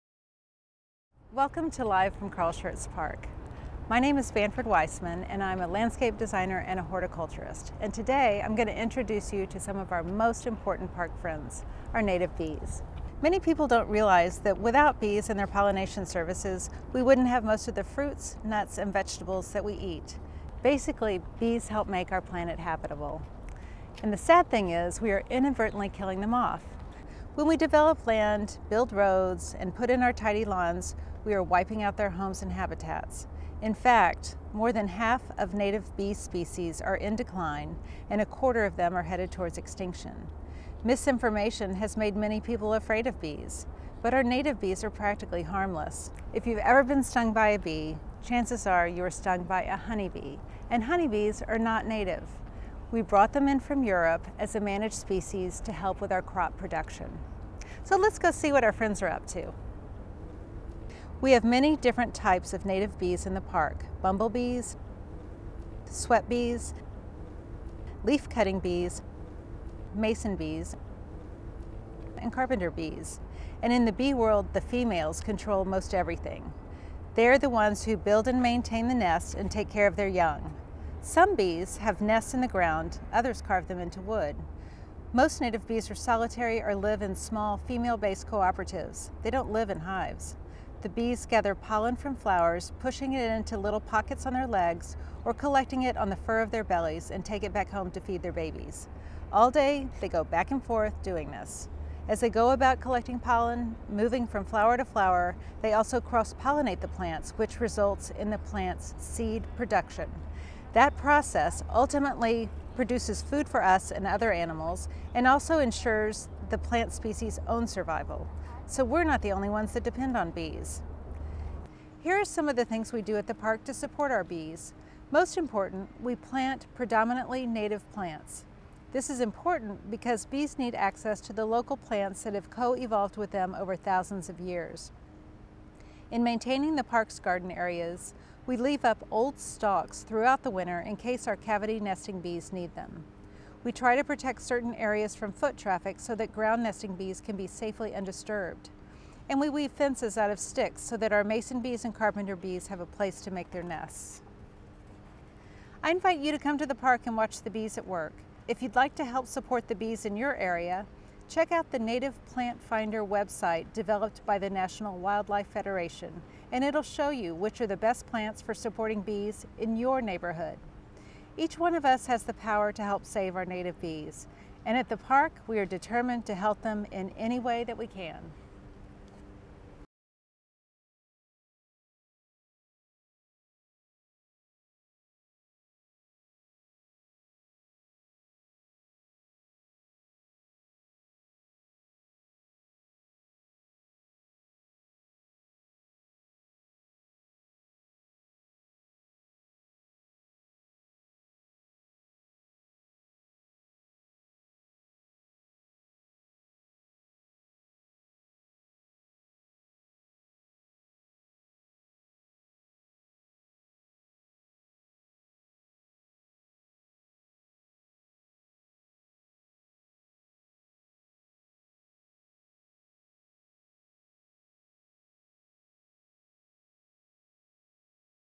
Live from Carl Schurz Park: Native Bees
This video is focused on Native Bees and is introduced by our Master Horticulturist, Banford Weissmann who recently worked with The American Museum of Natural History to document our bee population.